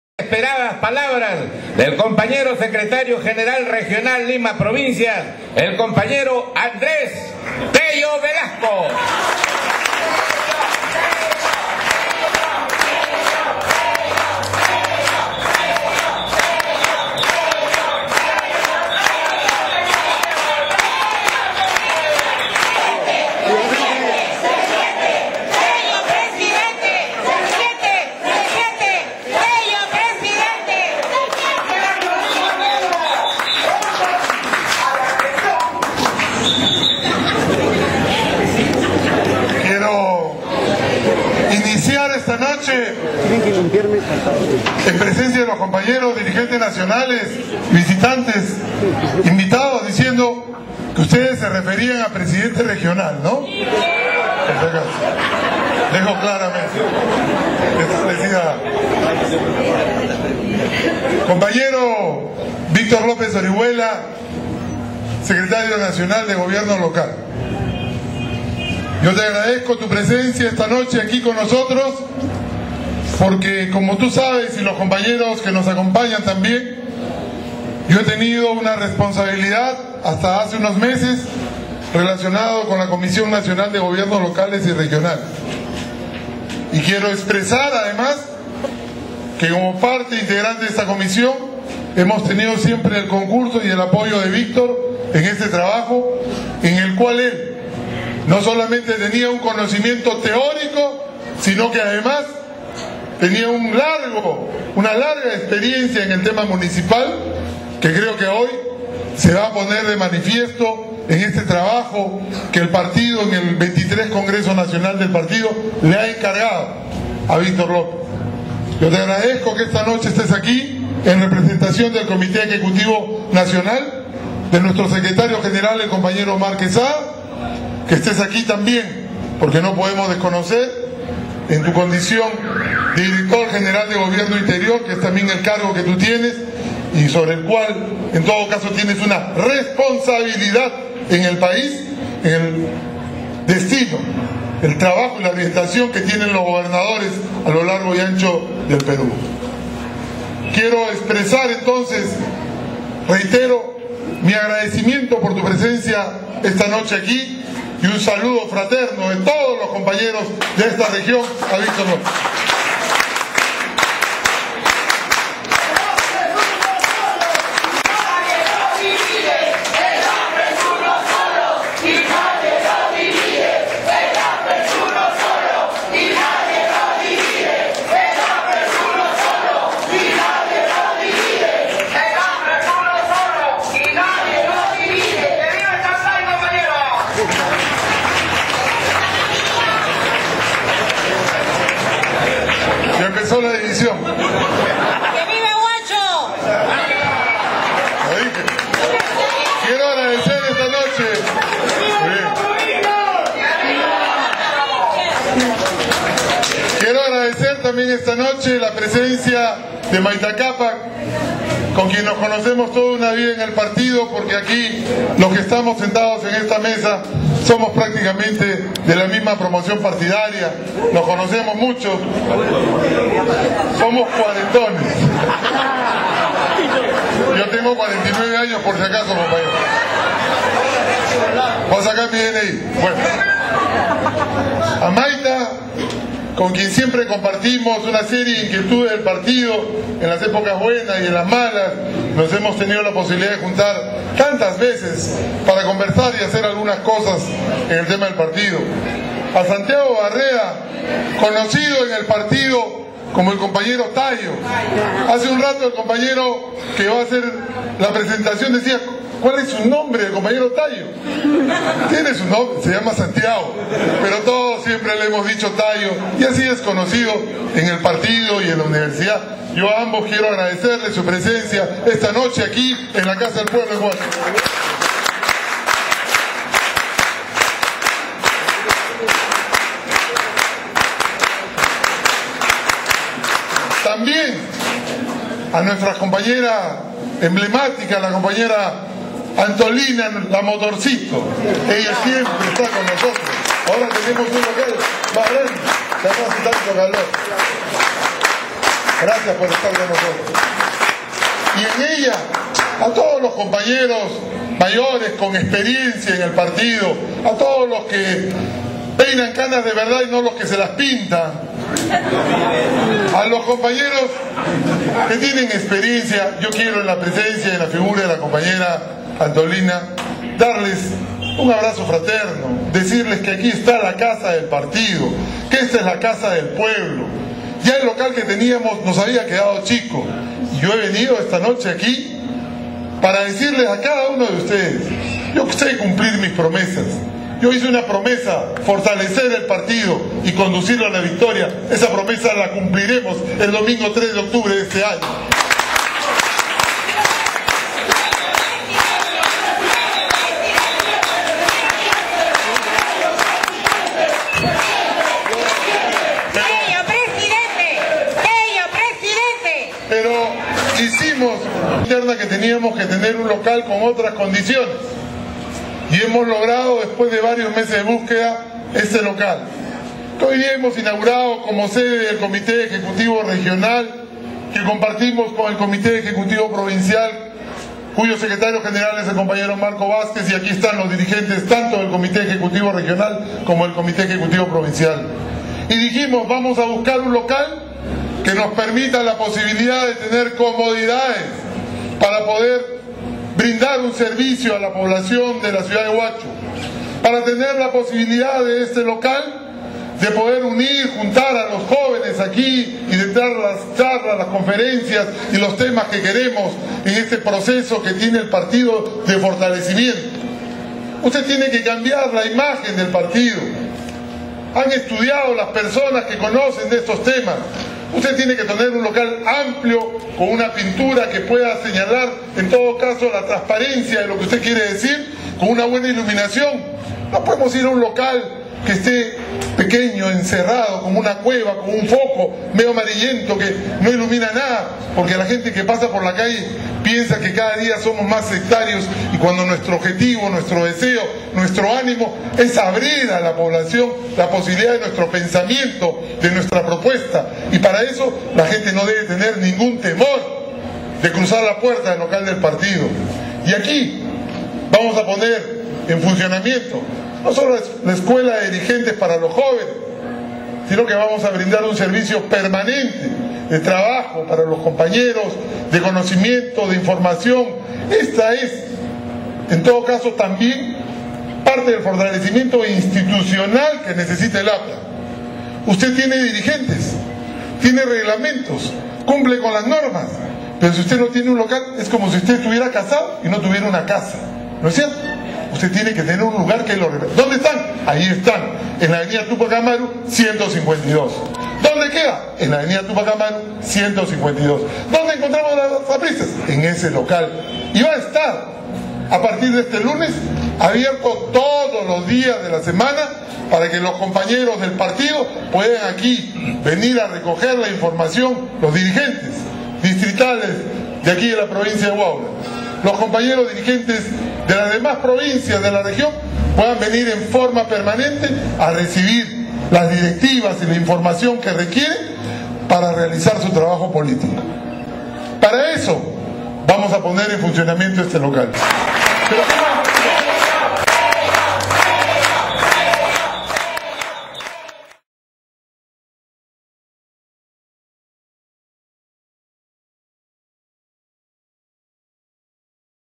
DISCURSO ATV PARTE 1.wmv
Inauguración de la Casa del Pueblo Región Lima Provincias, realizada en Huacho. 18MARZO2010